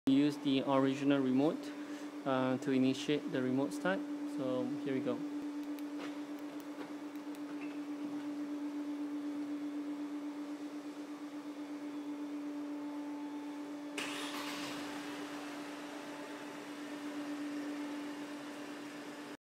Mercedes Remote Start
Remote start for Mercedes. Secure and convenient. Uses your existing key remotes. Doesn't need to 'sacrifice' any of your existing remote or disable any of your factory fitted security system.